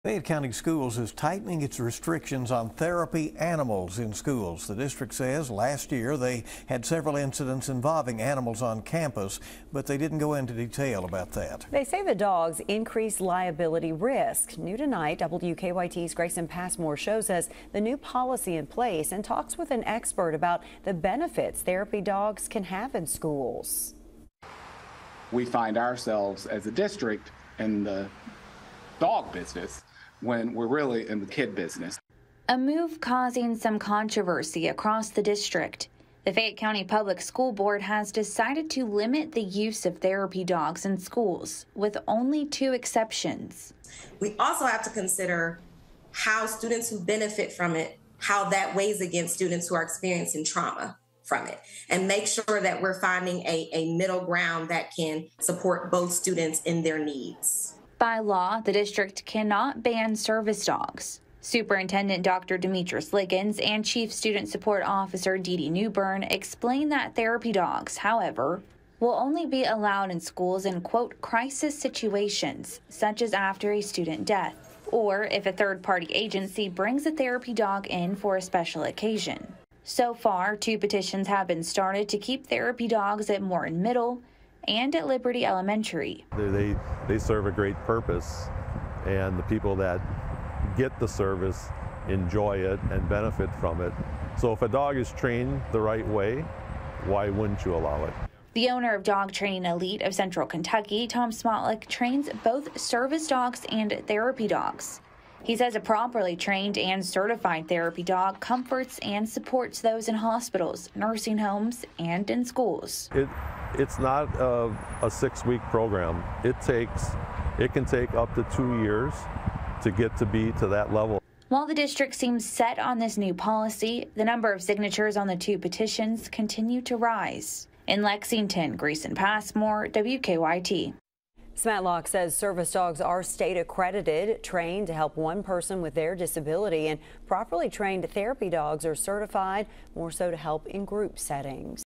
WATCH | FCPS policy tightens restrictions on therapy animals in schools
A move causing some controversy across the district, the Fayette County Public School Board has decided to limit the use of therapy dogs in schools.